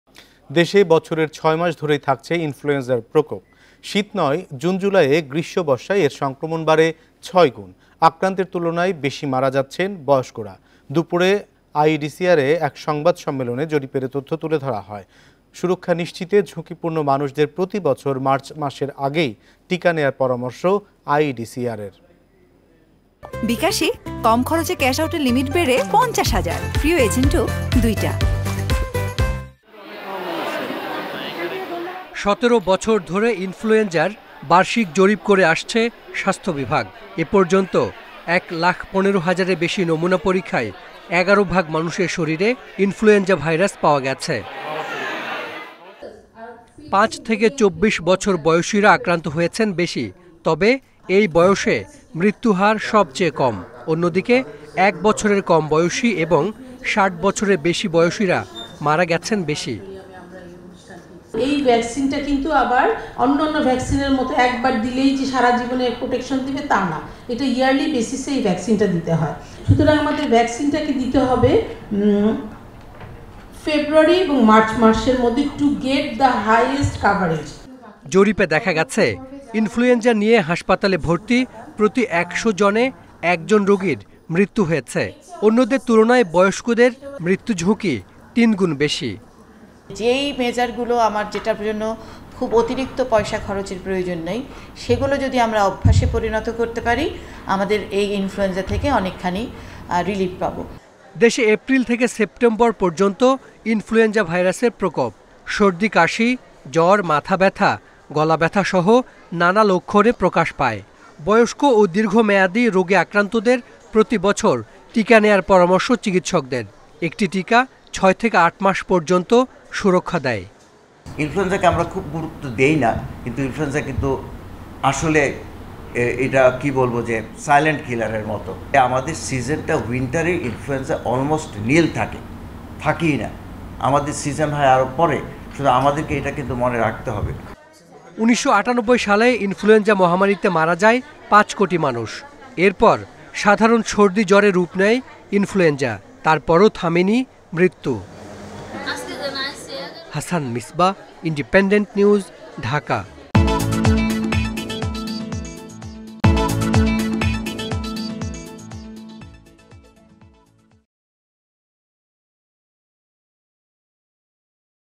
ইনফ্লুয়েঞ্জার প্রতিরোধে প্রতি বছর মার্চের আগে টিকা নেয়ার পরামর্শ | influenza Survey | Independent TV
দেশে বছরের ৬ মাস ধরেই থাকছে ইনফ্লুয়েঞ্জার প্রকোপ। শীত নয়, জুন-জু্লাইয়ে গ্রীষ্ম-বর্ষায় এর সংক্রমণ বাড়ে ৬ গুণ। আক্রান্তের তুলনায় বেশি মারা যাচ্ছেন বয়স্করা। বুধবার দুপুরে আইইডিসিআরে এক সংবাদ সম্মেলনে জরিপের এ তথ্য তুলে ধরা হয়। সুরক্ষা নিশ্চিতে ঝুঁকিপূর্ণ মানুষদের প্রতি বছর মার্চ মাসের আগেই টিকা নেয়ার পরামর্শ আইইডিসিআরের।